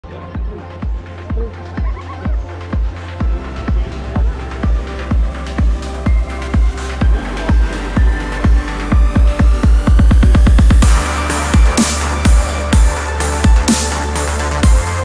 Aaron and Archer at the Jayco booth/ Tampa RV SuperShow 2022.
That's one smart dog! Time to teach our Boxer these moves!
Thought we posted this to YT before but apparently not. We visited the @Jayco booth at the 2022 Tampa RV SuperShow multiple times on multiple days, just to the kids could catch a glimpse of Archer (who cares about Aaron.. jk!)  Jayco had soooo many great RVs there, it was hard to pick a favorite. With 3 kids, we definitely lean towards anything with a bunk house and closing door though!